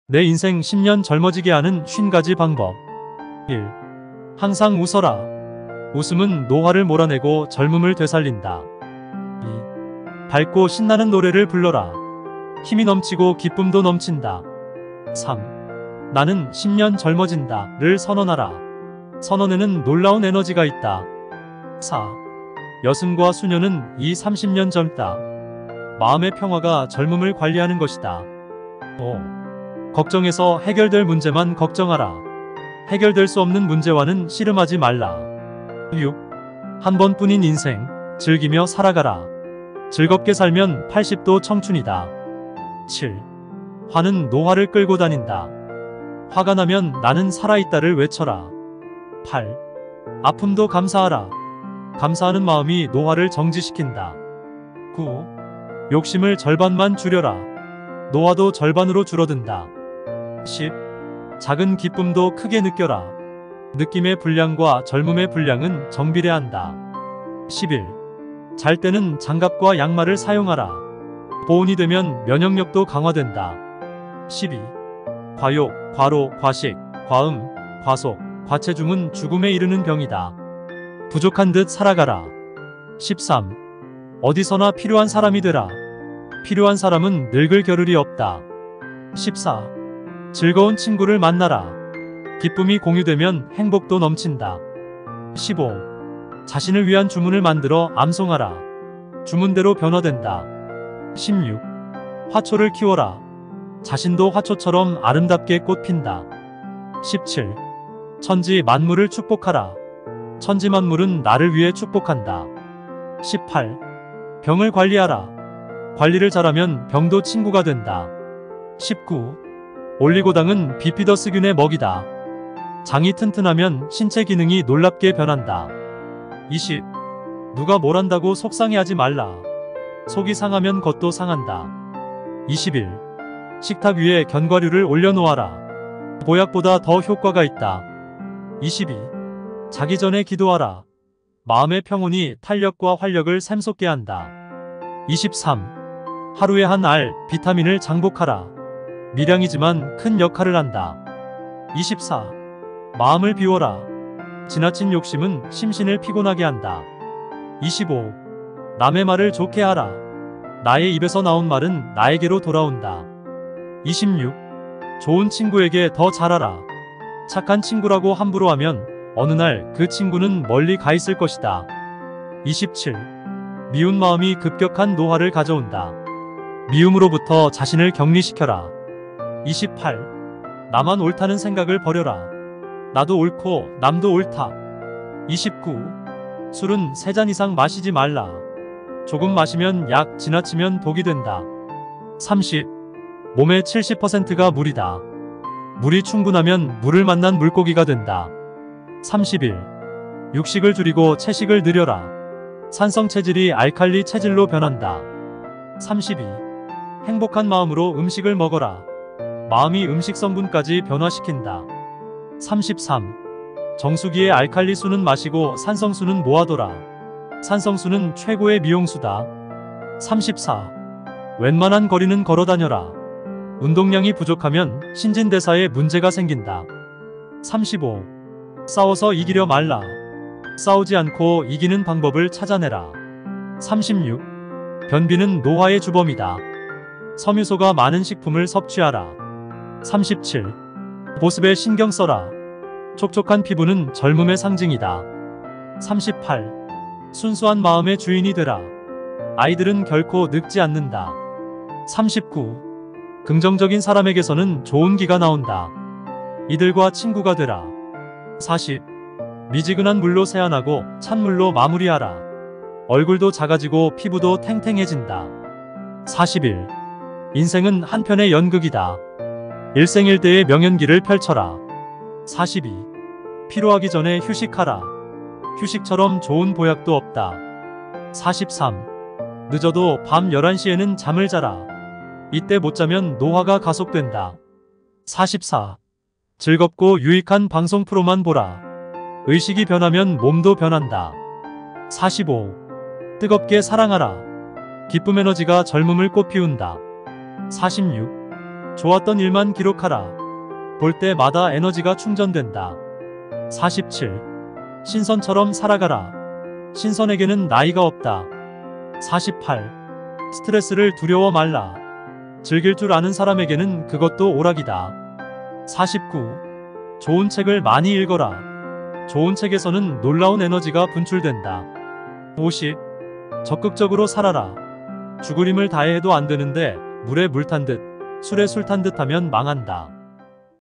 내 인생 10년 젊어지게 하는 50가지 방법
내 인생 10년 젊어지게 하는 50가지 방법을 담은 영상입니다.
반복해서 청취하시면 행복과 인생의 지혜를 얻으실 겁니다.
이 영상으로 인해 여러분들 삶이 행복하기를 바랍니다.